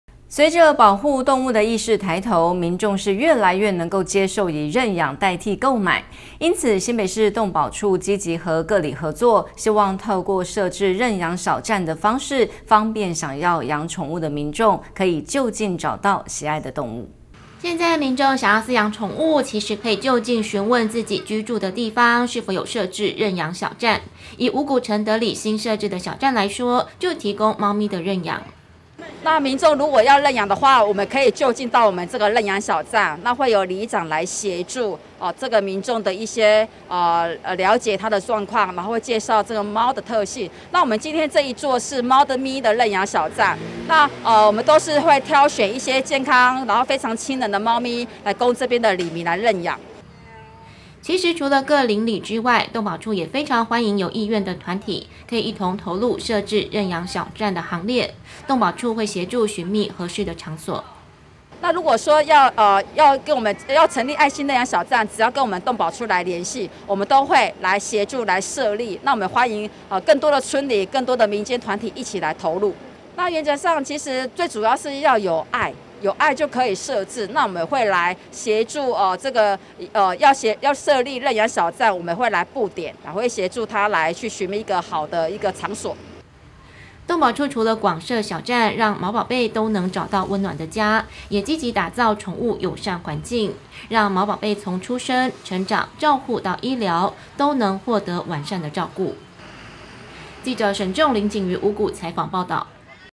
天外天新聞1110627 02積極設置小棧 提供民眾認養
隨著保護動物的意識抬頭，民眾越來越能接受以認養代替購買，因此新北市動保處積極和各里合作，希望透過設置認養小棧的方式，方便想要養寵物的民眾，可以就近找到喜愛的動物。